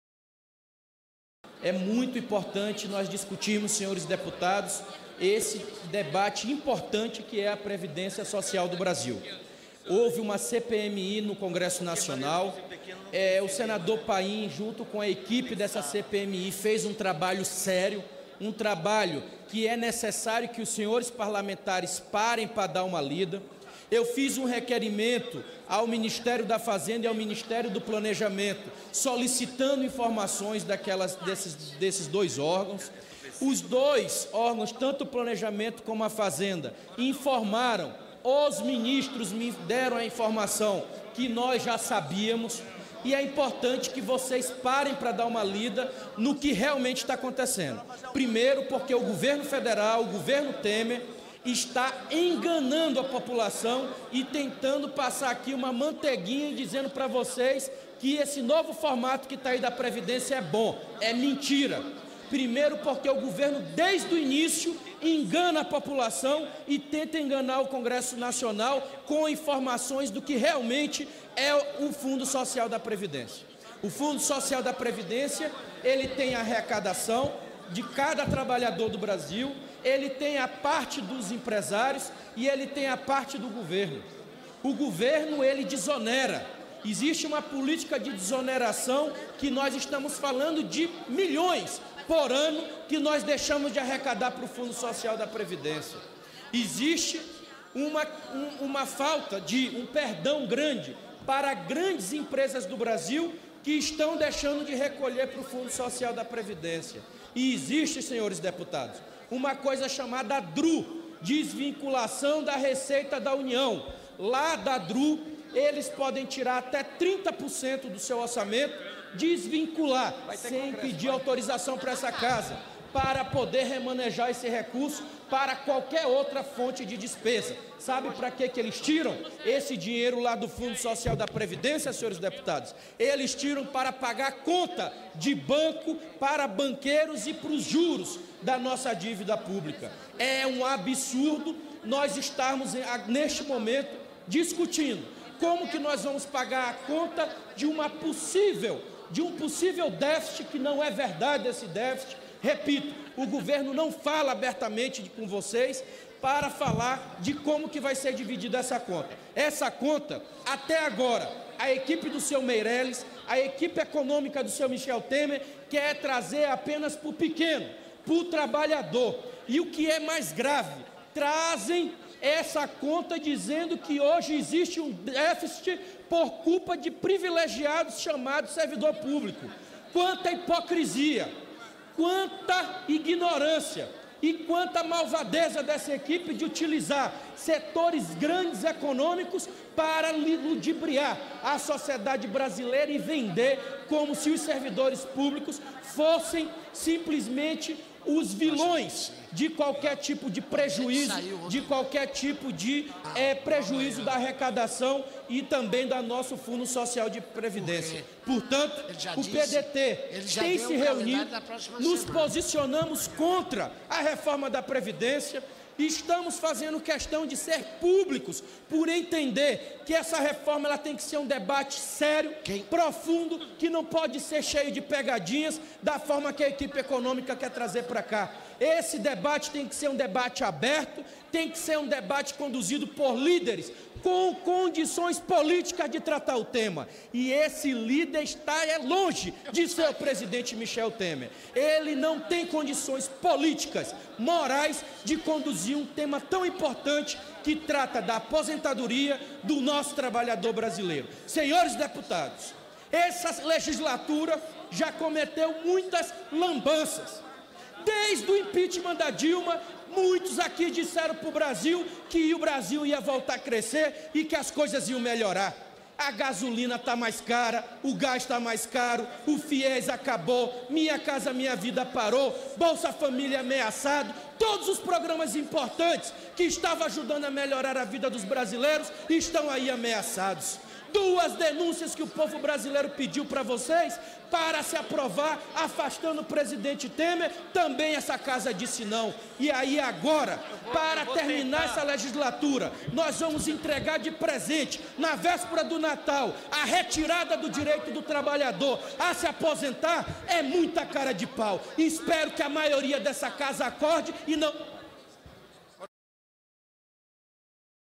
PDT contra a Reforma da Previdência
O líder do PDT na Câmara novamente colocou o posicionamento do partido contra a Reforma da Previdência e destacou os resultados da CPMI do Congresso Nacional, coordenada pelo Senador Paulo Paim que demonstrou que a previdência brasileira é superavitária, e ainda a necessidade de um debate amplo, com líderes que tenham legitimidade para conduzir os rumos do futuro do país e a manutenção dos direitos dos trabalhadores.